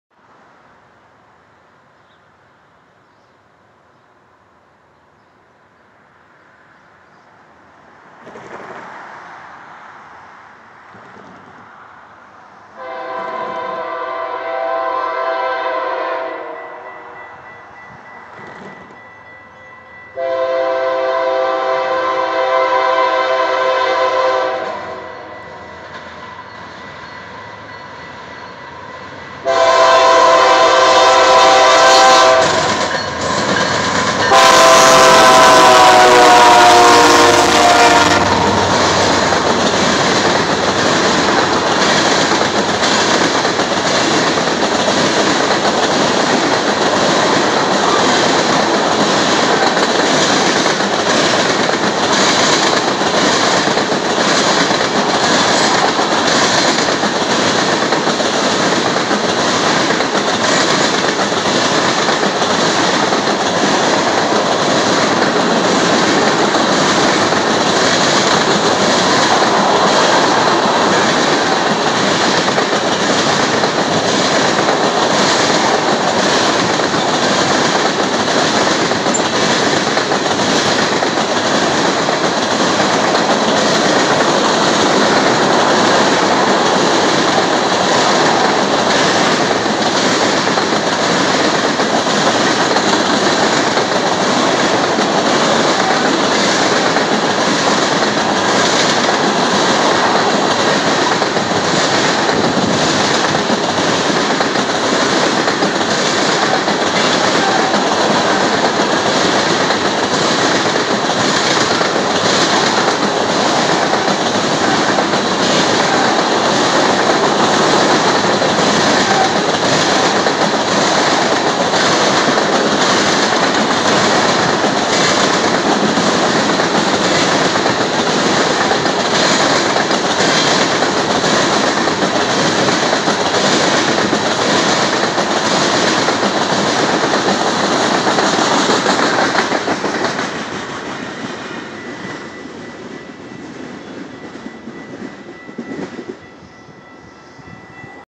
Canadian Pacific AC4400CWM 8116 westbound at Rondout, Illinois
Canadian Pacific AC4400CWM 8116 & ES44AC 8960 are seen powering a westbound ethanol train through Rondout, Illinois during the afternoon of April 24, 2021.